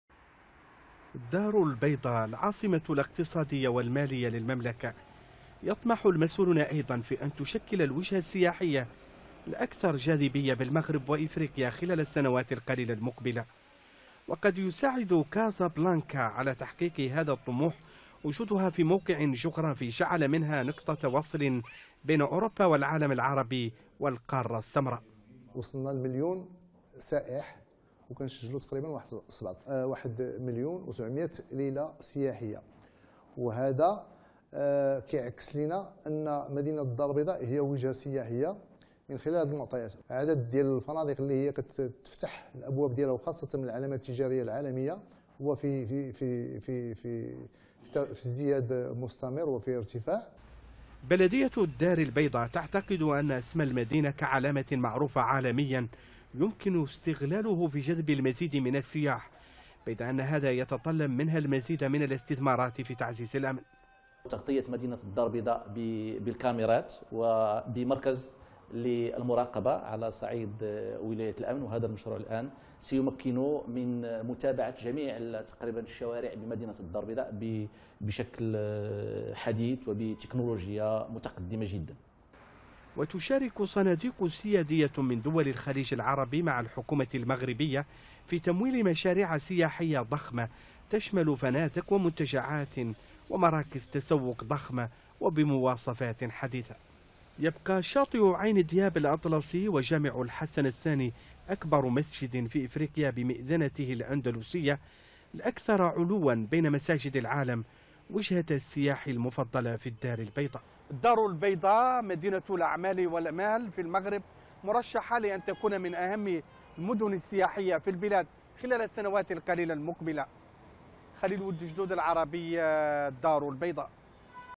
الدار البيضاء تسعى للتحول إلى مركز سياحي
تعرف مدينة الدار البيضاء كقطب اقتصادي ومالي كبير في افريقيا لكن المدينة تستقطب أيضا الكثير من السياح
ويطمح المسؤولون في أن تكون الدار البيضاء الوجهة السياحية الأولى في المغرب وافريقيا خلال السنوات المقبلة